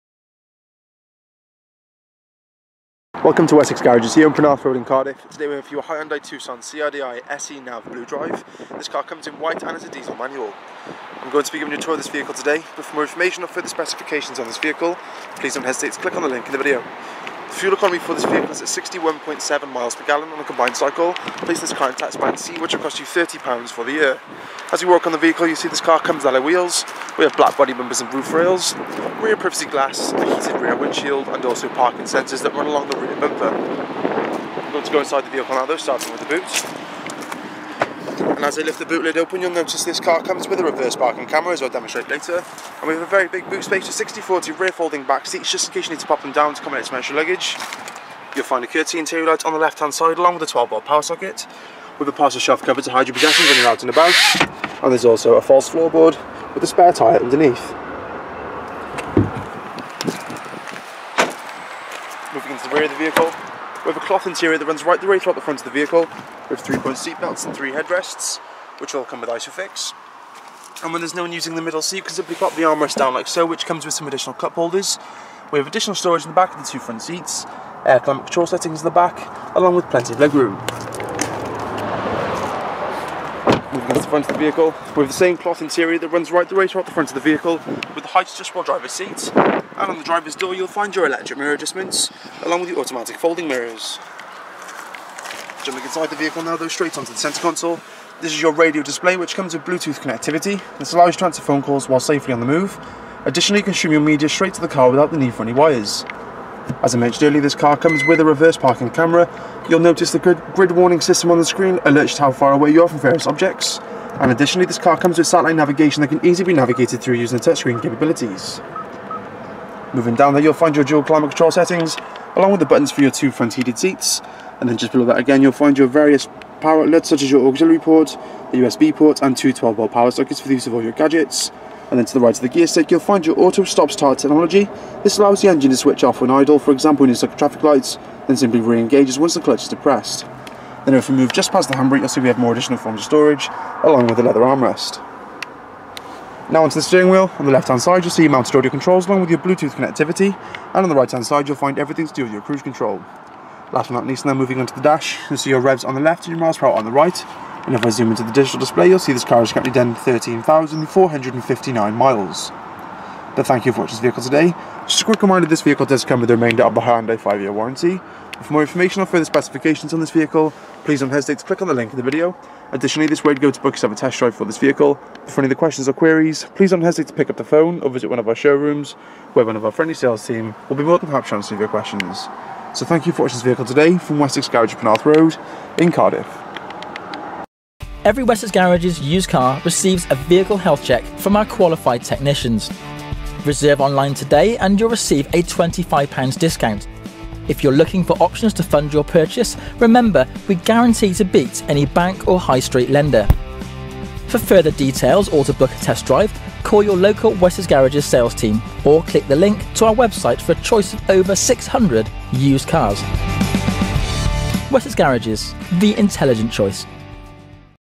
Wessex Garages Penarth Road Cardiff, Used, Hyundai Tucson BLUE DRIVE, DIESEL, MANUAL, CK66OHSRDI SE
This vehicle was first registered in 2016 and has 2 previous owners.